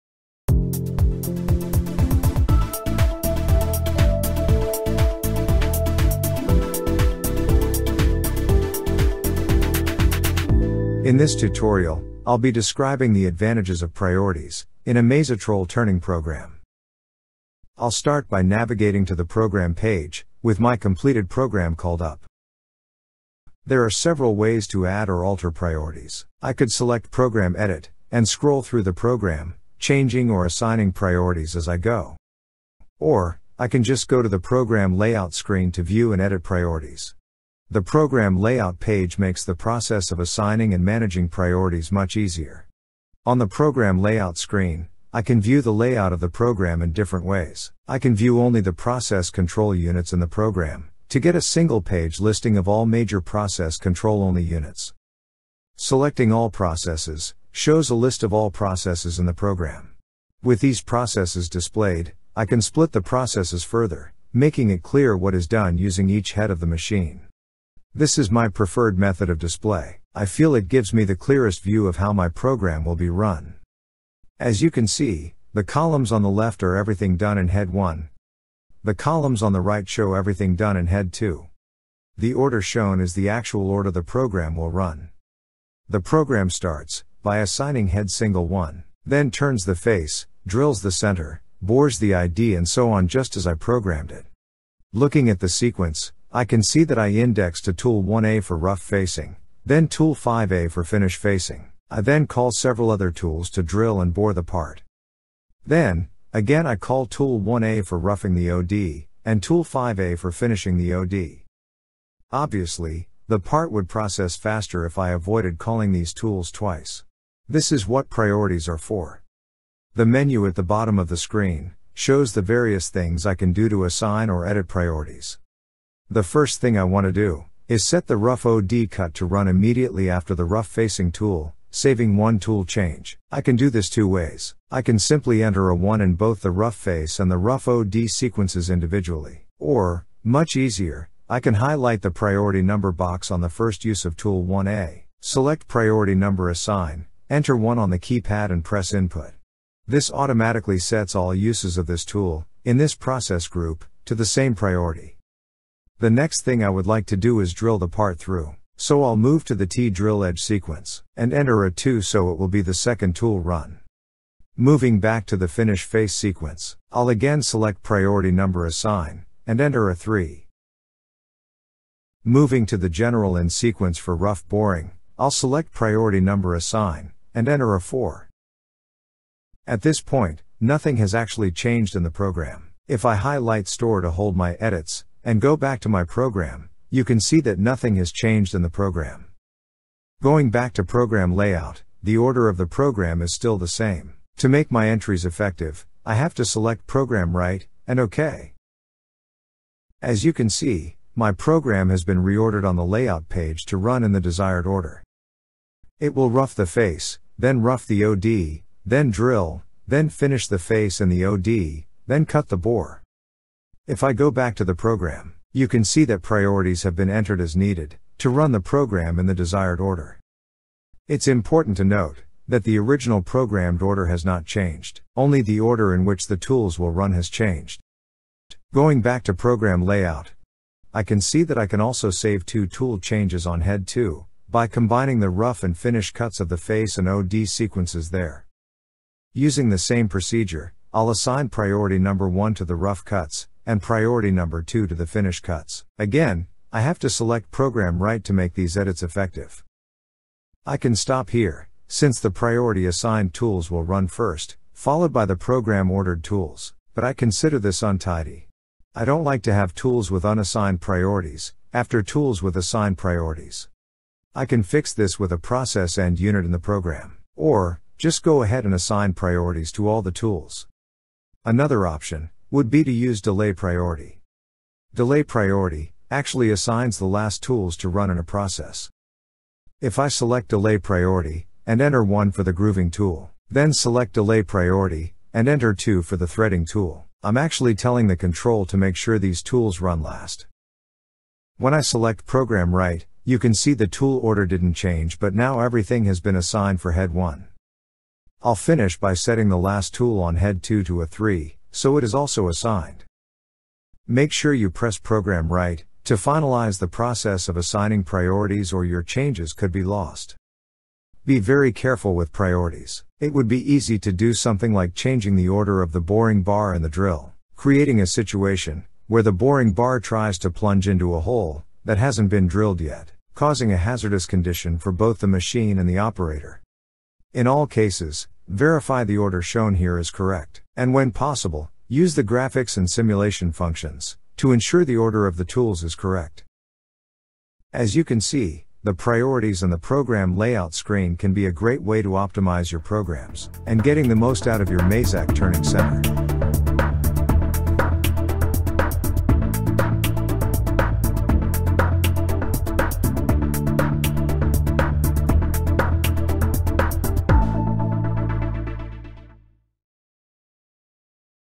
MAZATROL Programming Briefs: Priorities & Layout
In this tutorial learn the advantages of Priorities in a MAZATROL Turning Program.